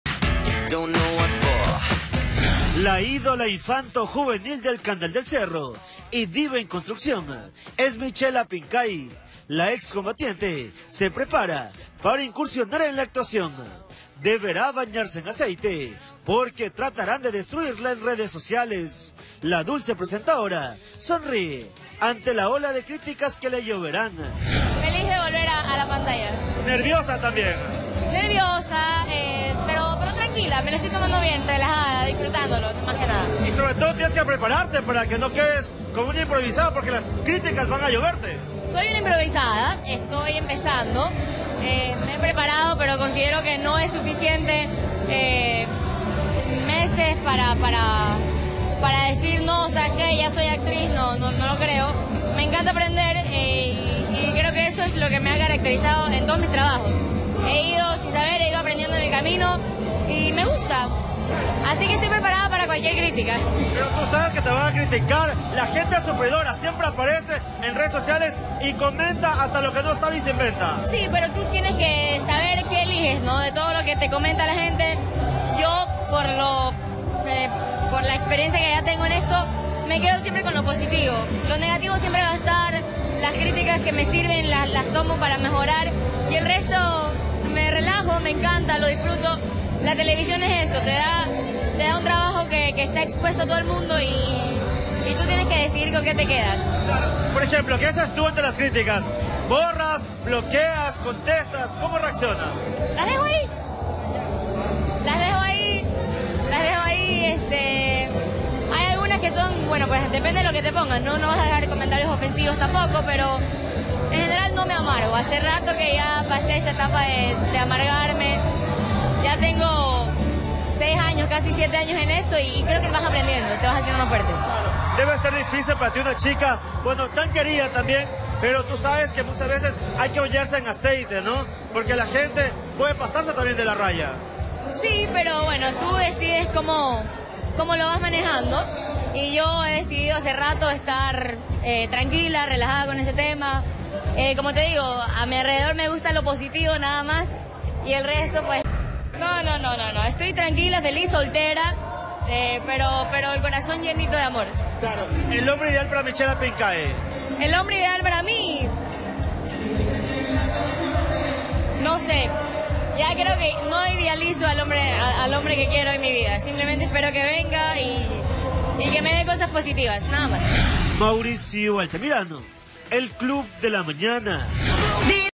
Así responde Michela Pincay a críticas en redes sociales
Le presentadora incursiona en la actuación

Michela Pincay afirmó que está empezando y ha trabajado duro en su preparación. Afirmó que está emocionada por este reto y toma clases de actuación.